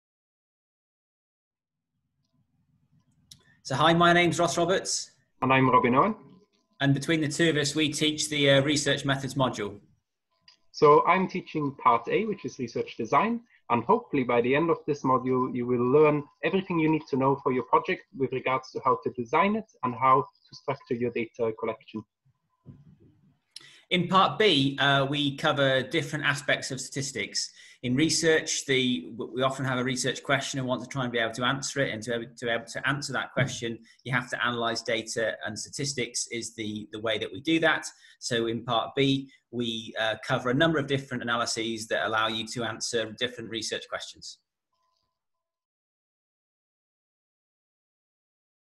Dr Robin Owen & Dr Ross Roberts - Research Methods Module. Sport, Health and Exercise Science.
Do you want to understand why, for example, some athletes perform better than others under pressure, why obesity is on the rise, how training and recovery influences performance or something else related to sport and physical activity? If so this module is for you! Research is fundamental to what we, as scientists, do and informs practice in the sport, health and fitness industry. Therefore, developing good research skills and being able to analyse scientific data are key skills to develop in order to support your successful career. The module is hands on and involves practical activities relating to data management, running data analyses, understanding different approaches to science, and developing your own research projects. These are real world graduate skills that will help make you more employable.

Ydych chi eisiau deall pam, er enghraifft, bod rhai athletwyr yn perfformio'n well nag eraill sydd dan bwysau, pam mae gordewdra ar gynnydd, sut mae hyfforddiant ac adferiad yn dylanwadu ar berfformiad neu rywbeth arall sy'n gysylltiedig â chwaraeon a gweithgaredd corfforol? Os felly mae'r modiwl hwn ar eich cyfer chi! Mae ymchwil yn sylfaenol i'r hyn yr ydym ni, fel gwyddonwyr, yn ei wneud ac yn llywio ymarfer yn y diwydiant chwaraeon, iechyd a ffitrwydd. Felly, mae datblygu sgiliau ymchwil da a gallu dadansoddi data gwyddonol yn sgiliau allweddol i'w datblygu er mwyn cefnogi'ch gyrfa lwyddiannus. Mae'r modiwl yn ymarferol ac yn cynnwys gweithgareddau ymarferol yn ymwneud â rheoli data, cynnal dadansoddiadau data, deall gwahanol ymagweddau at wyddoniaeth, a datblygu eich prosiectau ymchwil eich hun. Mae'r rhain yn sgiliau graddedigion yn y byd go iawn a fydd yn helpu i'ch gwneud chi'n fwy cyflogadwy.